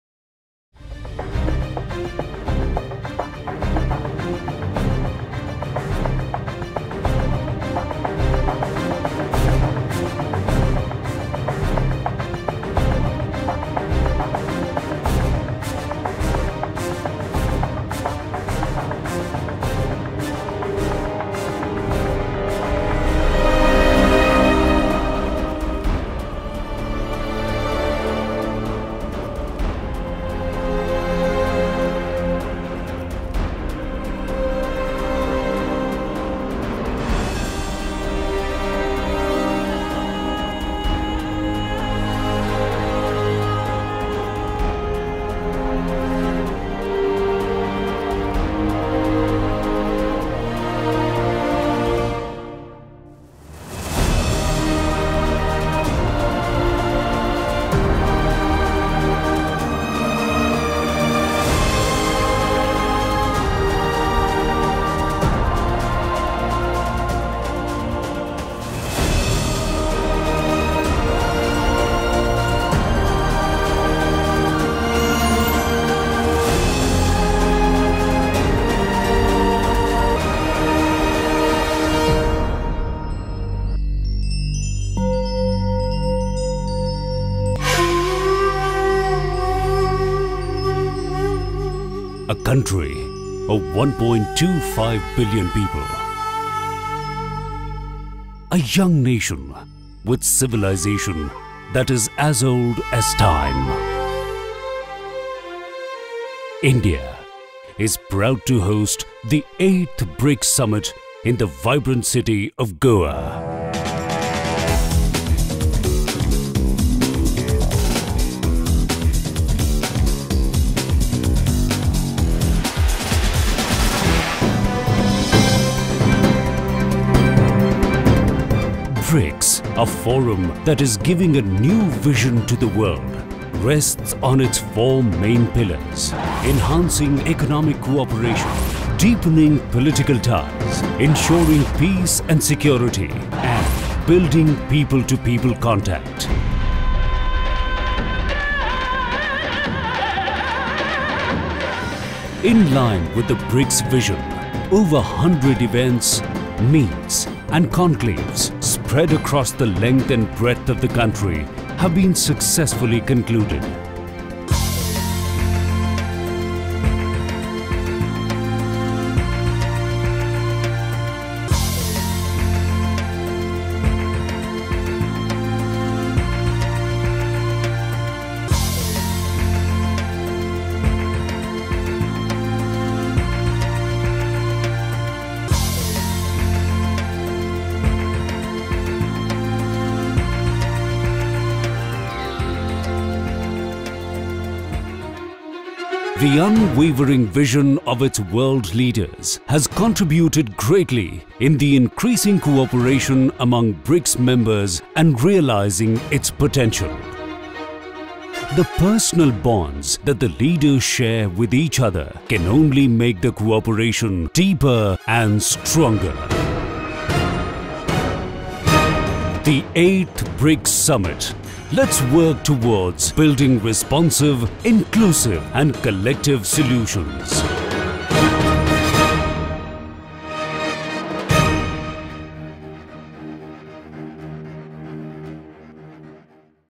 BRICS 2016
India is hosting the 8th BRICS Summit in Goa from 15-16 October, 2016. This is a short introductory film for the Summit.